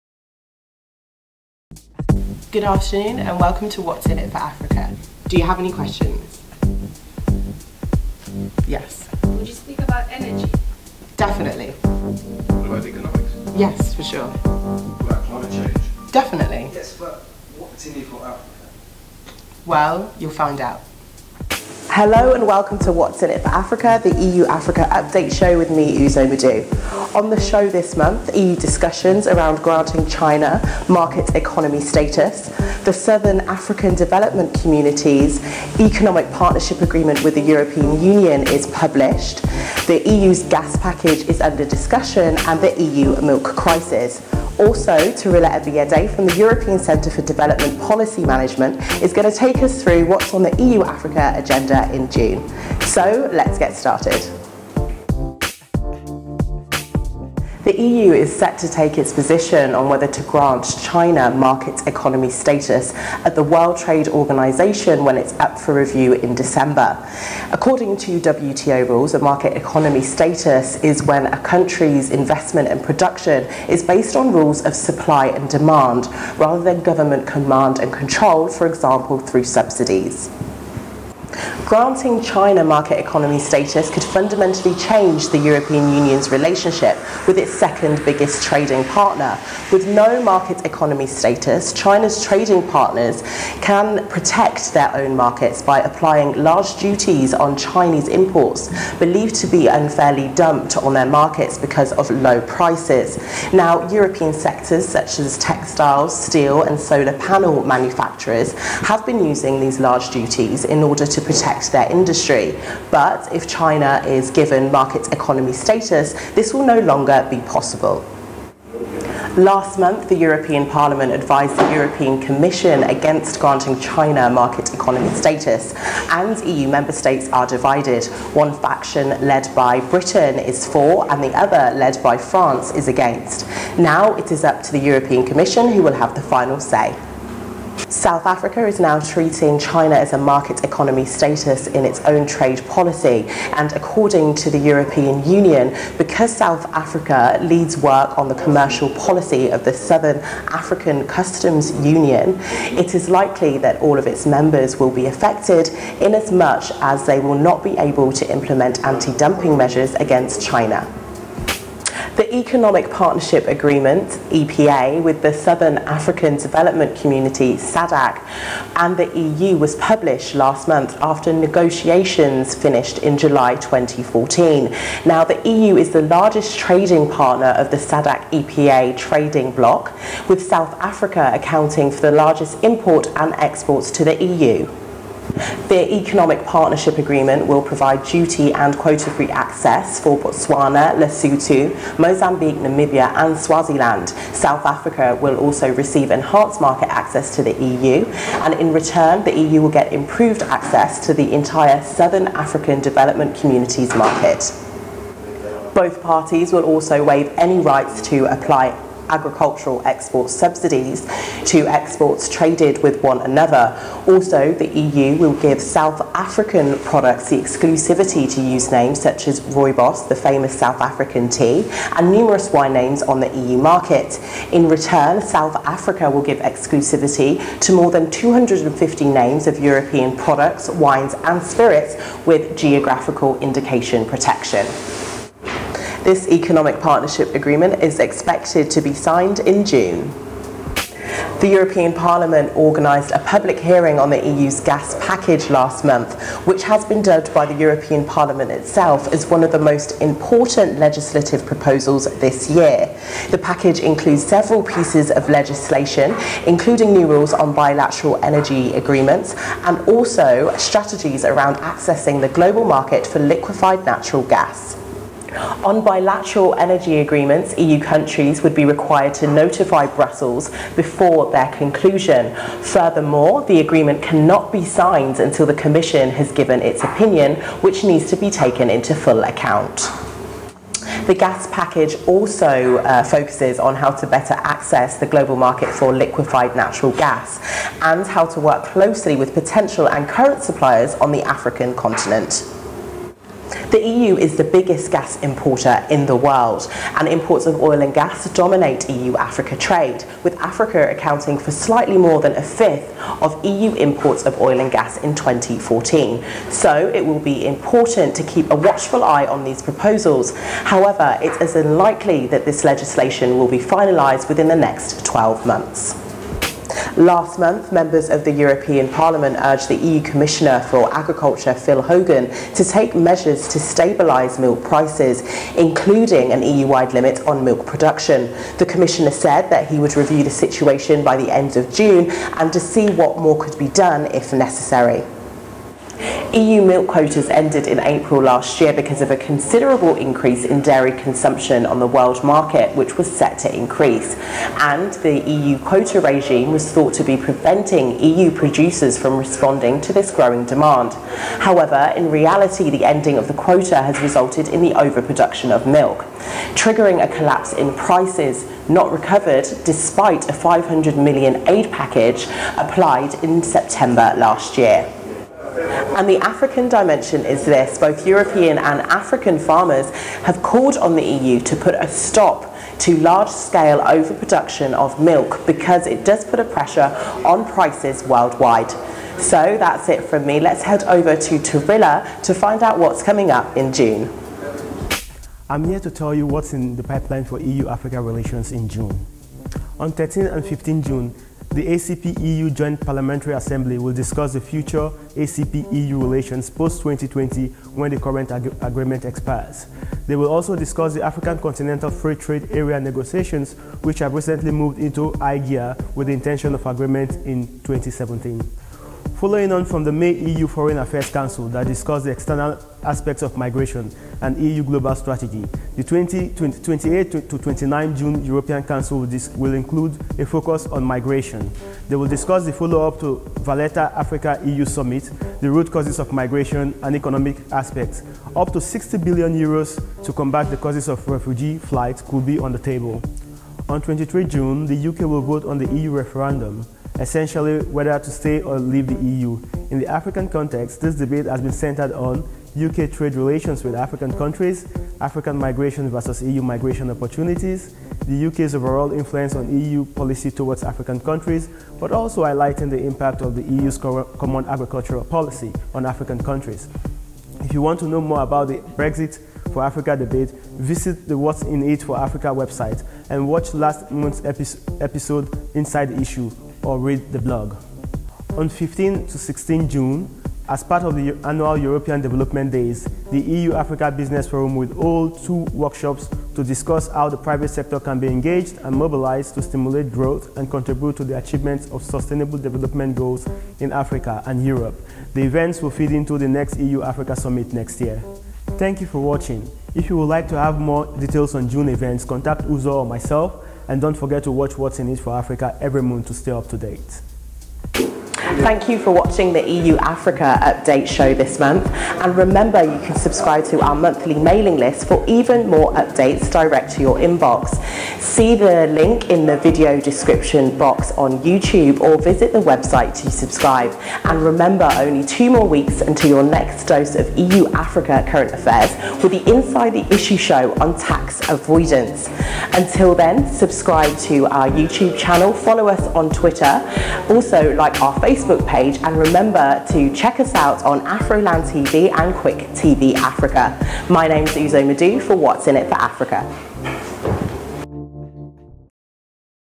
June EU-Africa Update: China's MES, SADC EPA, Gas Package, EU Milk Crisis
On the show this month:

• EU discussions around granting China Market Economy Status (MES) at the WTO
• The Southern African Development Community’s (SADC) Economic Partnership Agreement (EPA) with the EU is published
• The EU Gas Package proposals are under discussion and;
• The EU Milk Crisis

Also, Tarila Ebiede from the European Centre for Development Policy Management (ECDPM) will be letting us know what is happening in June!